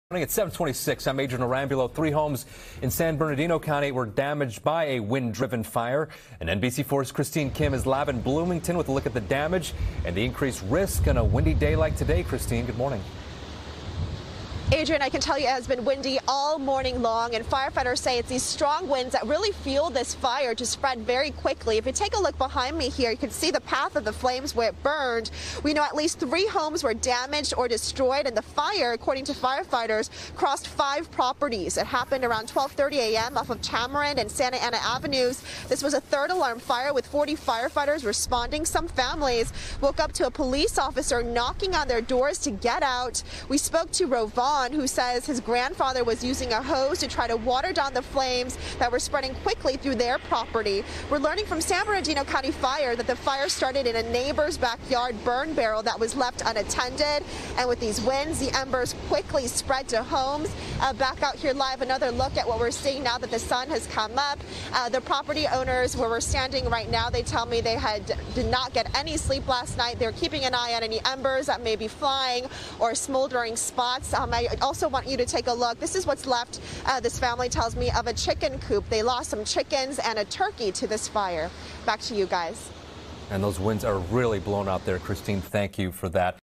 02.25.21-NBCLA 730A-Wind Driven Fire Spreads to Three Homes
SBCoFD responded to the 10700 block of Tamarind Avenue for a report of a backyard on fire.Strong winds, with gusts of up to 50 mph, fueled the fire. It spread to two other homes and burned approximately two acres of brush before crews knocked the blaze down within 90 minutes. San Bernardino County Fire Department said that investigators were able to track the cause and origin of the fire to a nearby burn barrel where a non-permitted fire was left unattended Winds sent embers to brush and homes.